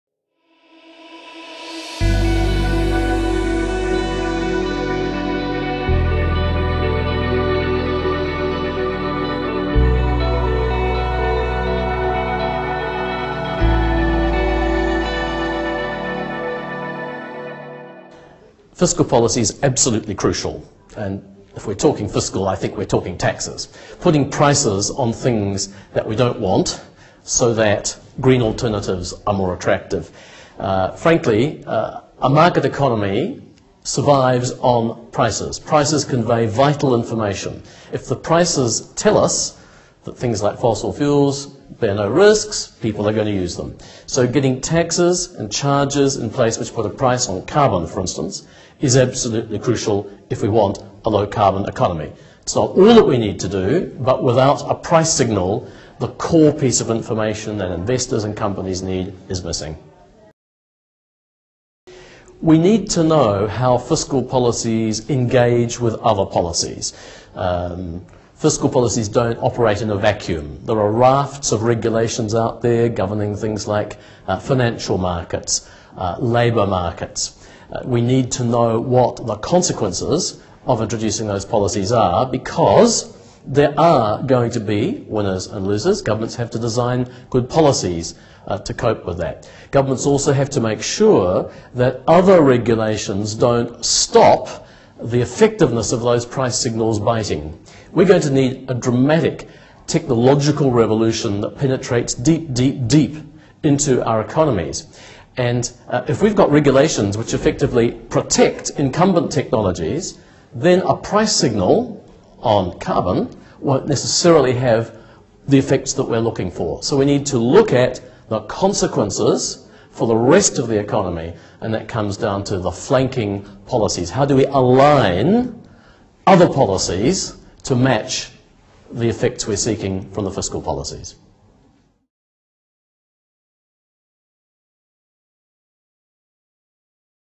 Simon Upton on the importance of fiscal policy to the green economy transition
Simon Upton, Director of the Environment Directorate at the Organisation for Economic Co-operation and Development, discusses the role of fiscal policy in the green economy transition and the research that is still needed to best use fiscal policy as a green growth tool.  This interview was conducted at the GGKP Third Annual Conference (January 29-20, 2015) on the topic of "Fiscal Policies and the Green Economy Transition: Generating Knowledge – Creating Impact".